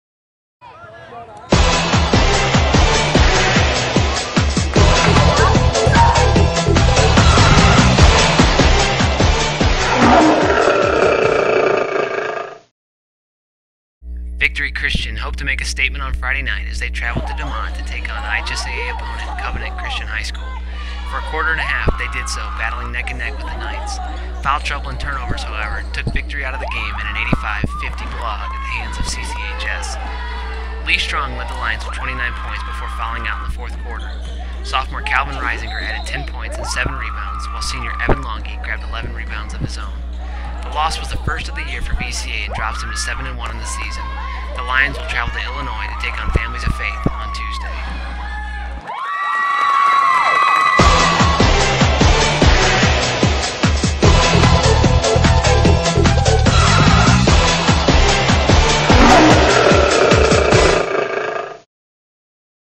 VCA @ Covenant Highlights
Victory Christian Academy squares off with Covenant Christian High School in non-conference basketball match-up.